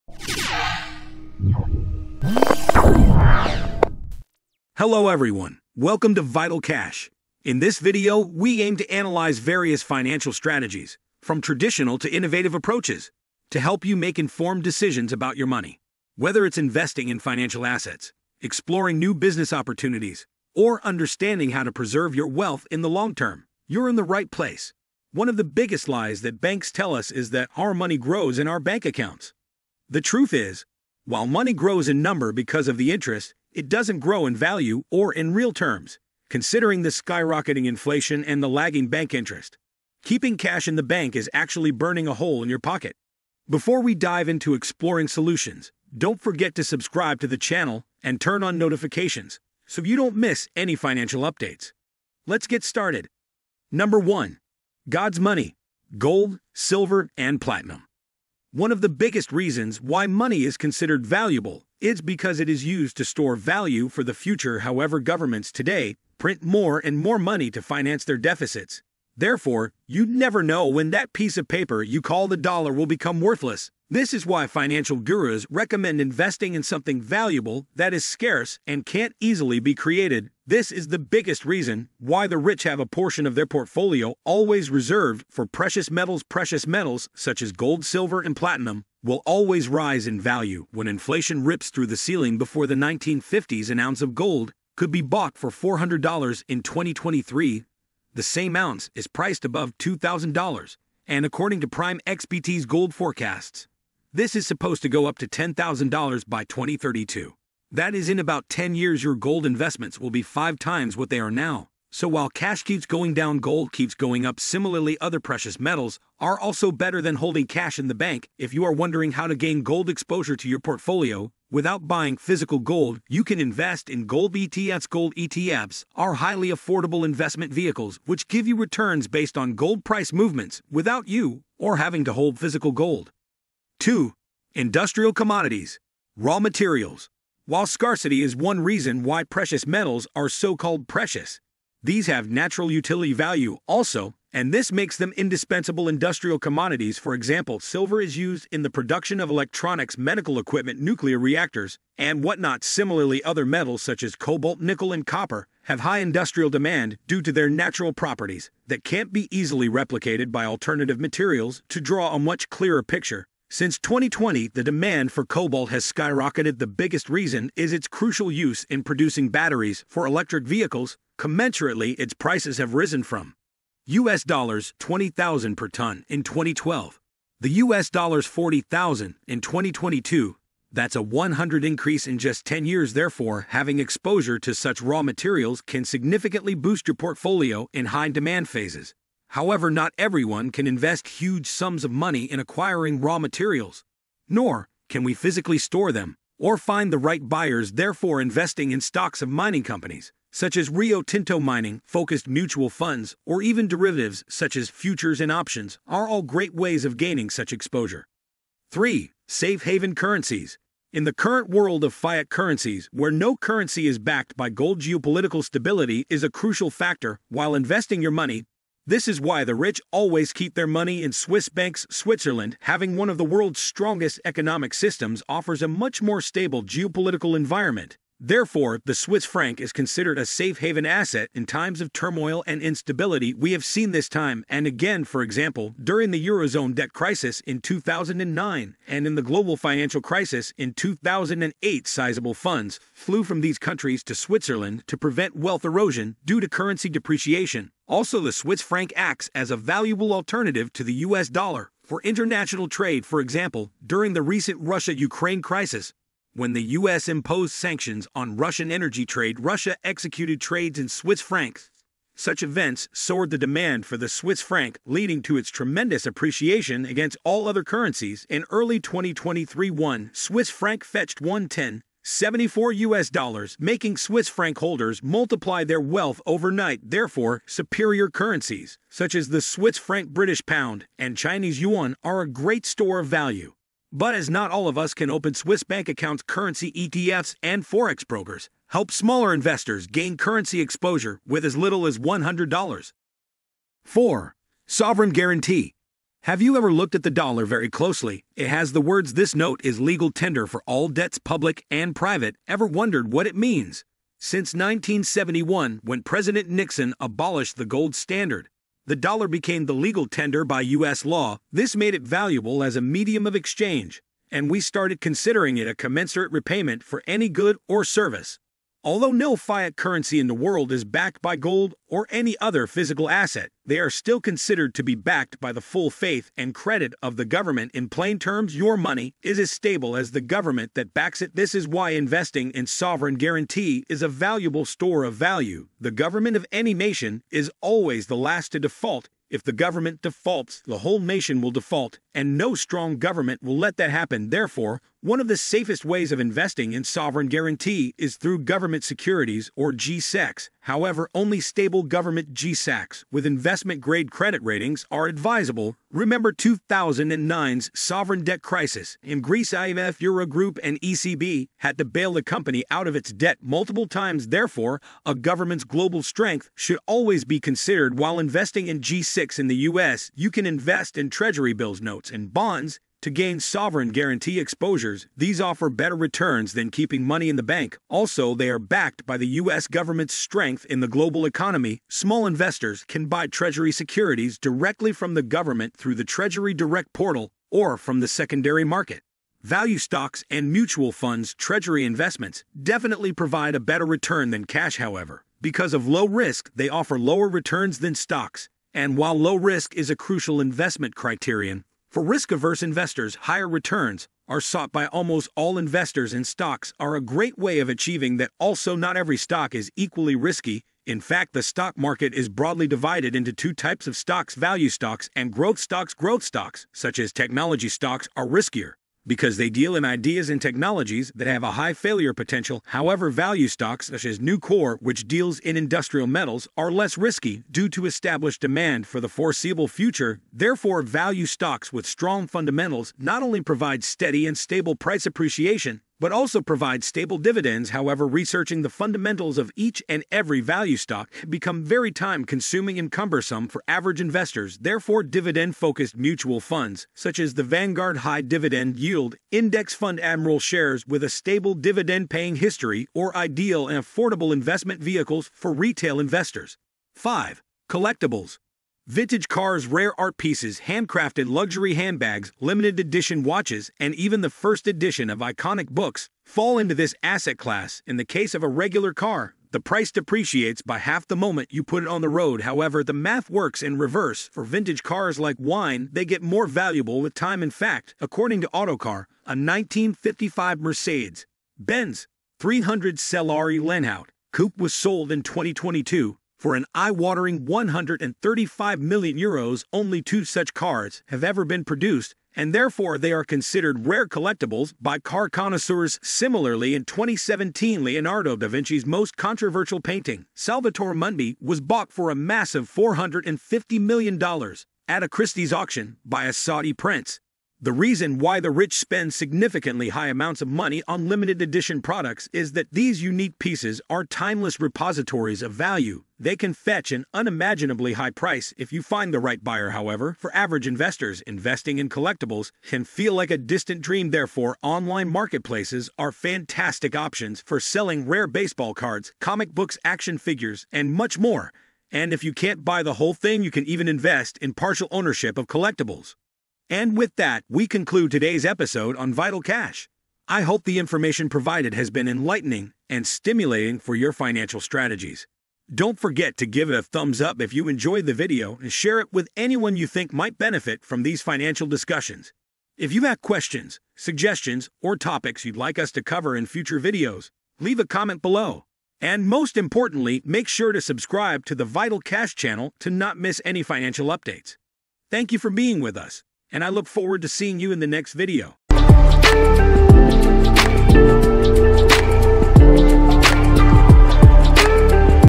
How To Stop Burning Your Savings In The Bank!
📽️In this video we'll explore a comprehensive approach to financial management, covering wealth protection to strategic investments. 💲It's going to be an insightful journey through key themes to optimize your wealth and make informed financial decisions. Don't miss the chance to gain valuable financial insights! 📈🚀

🕒TIMESTAMPS🕒
0:00 | Intro
0:55 | God's Money
2:16 | Industrial Commodities
3:30 | Safe Haven Currencies
4:56 | Sovereign guarantee
7:38 | Collectibles